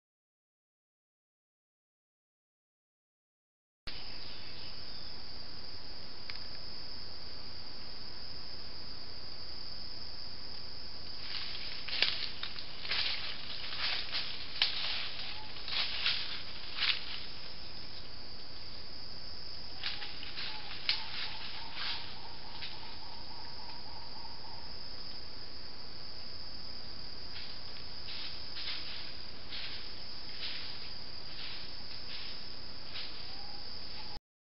Last chimps (Pan troglodytes ellioti) in Bamenda Highlands, Cameroon
cameratraps donated by Liberec ZOO